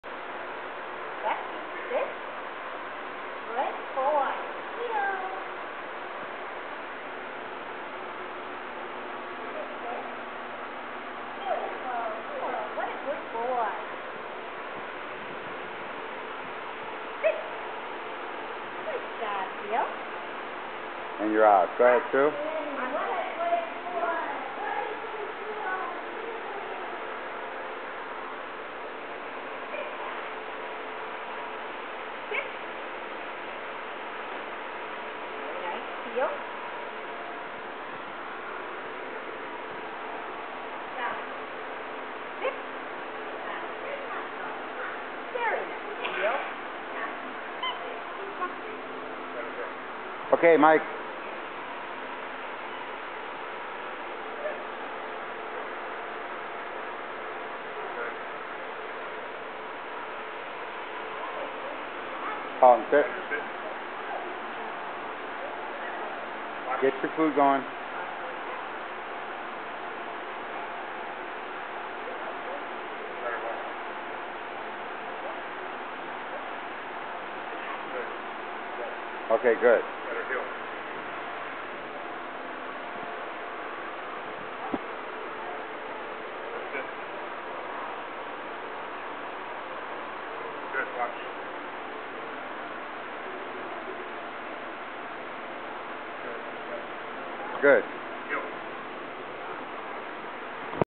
Allan Ross Dog Training
Group obedience on 7/31/2010.  We practice figure 8's around bowls of food.